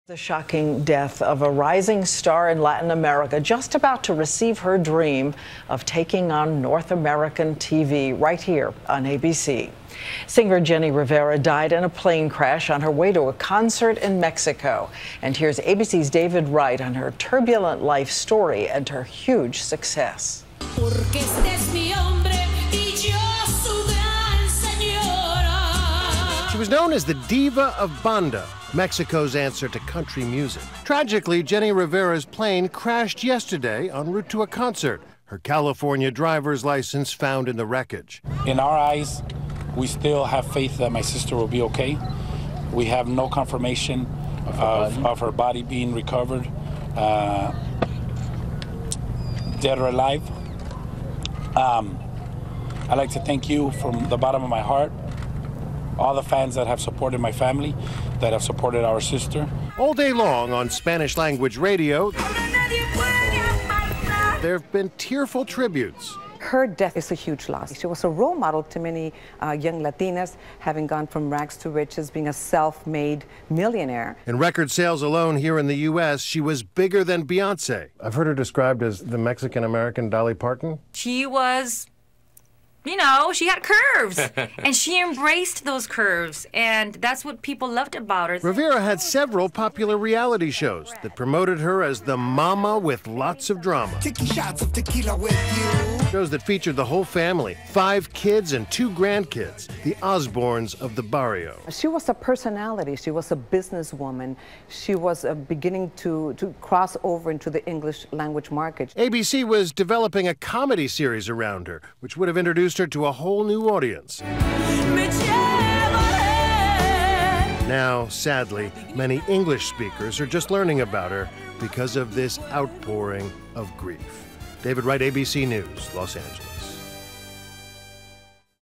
Jenni Rivera Dies in Plane Crash
Rivera, a rising star in Latin America, was about to make her big debut on American TV.